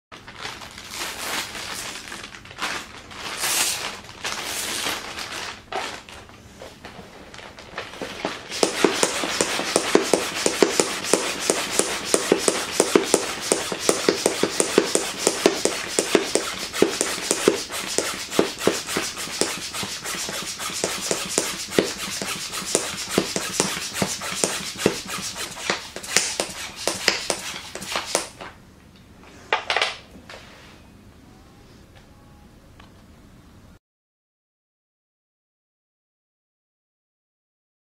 Vacuum Bag Hand Pump vs Hulk
Try saving your baggage space with this awesome technique. Your way to pack things up will be changed forever.